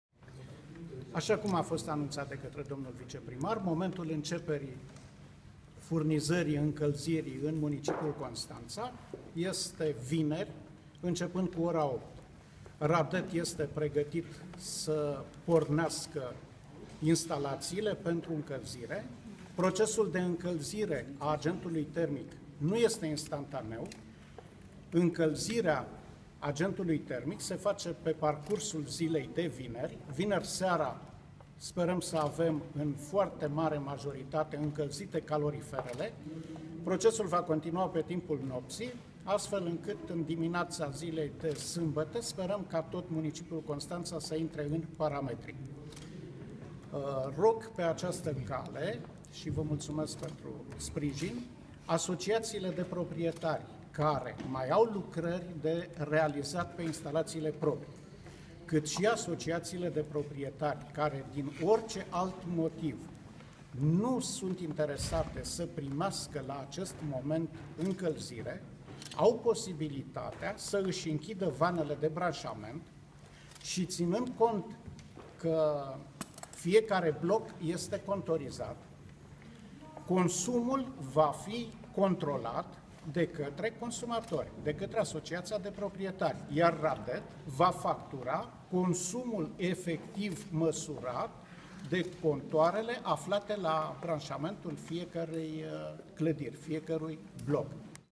ctnews ro | Liviu Popa anunta prima zi de incalzire a caloriferelor la Constanta
Liviu Popa anunta prima zi de incalzire a caloriferelor la Constanta